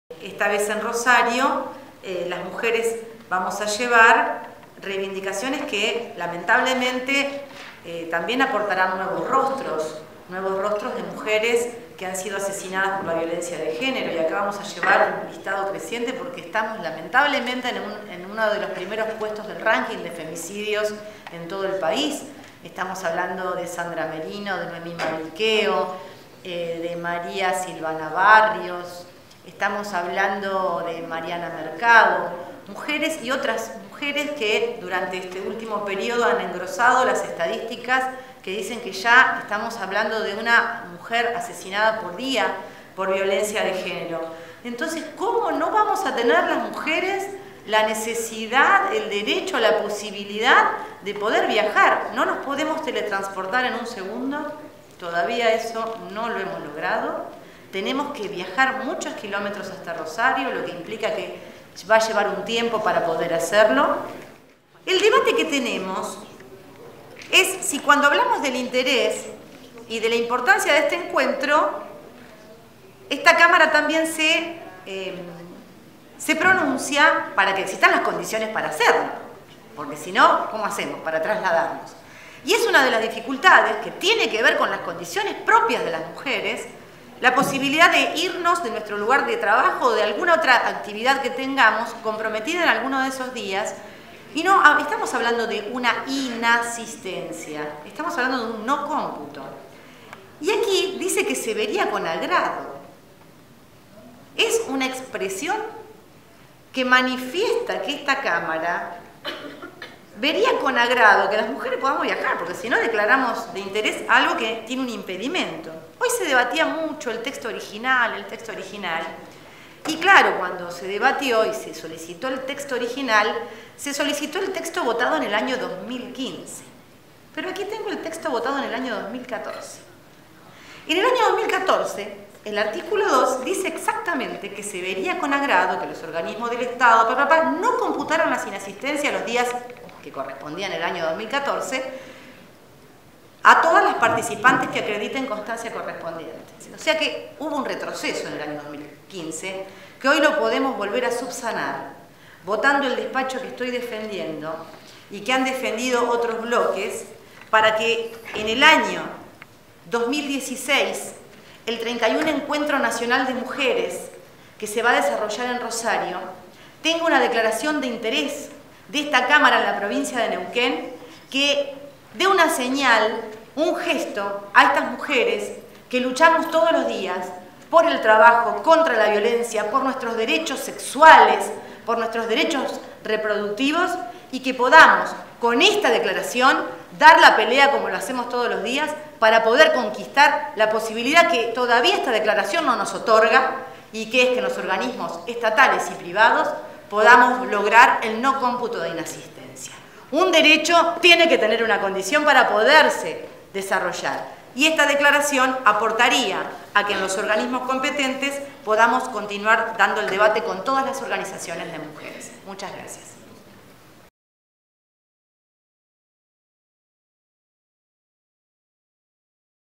Logramos la declaración de interés del Encuentro Nacional de Mujeres en Neuquén
Encuentro Nacional de Mujeres: Logramos la declaración de interés por parte de la legislatura
Intenso debate y férrea oposición del MPN.

En la sesión de hoy, y luego de intenso debate, la legislatura provincial sancionó una declaración de interés por la realización del Encuentro Nacional de Mujeres en Rosario y por el no cómputo de la inasistencia a todas las compañeras que asistan.

La diputada Patricia Jure del Partido Obrero por el Frente de Izquierda expresó "El proyecto se puso a consideración de la Comisión que preside el MPN luego de meses de desconocer notas y pedidos reiterados de apoyo de diversas organizaciones. Finalmente sobre la hora, el MPN expresó sin pelos en la lengua su negativa a que la declaración inste a organismos públicos y privados a no computar la inasistencia para poder asistir. Incluso llegó a proponer que el Encuentro se dividiera en regiones para que las mujeres no deban faltar y las que no puedan ir por problemas económicos se resignen. Una declaración inadmisible contra las mujeres, en una provincia signada por los femicidios crecientes, la trata y la violencia de género".

"Defendimos un despacho que quedó en minoría en la Comisión, pero que en el recinto se transformó en mayoría, quedando el MPN aislado y perdiendo la votación. Como ocurrió otros años esta declaración debe debatirse en el cuerpo colegiado del CPE y otros organismos públicos y privados.
En el día del aniversario del asesinato de Noemí Maliqueo, esta declaración es un aporte para las mujeres que luchamos por nuestros derechos laborales, sexuales y reproductivos. Que enfrentamos el ajuste y la impunidad del Estado. Vamos a Rosario a buscar un plan de lucha", concluyó la diputada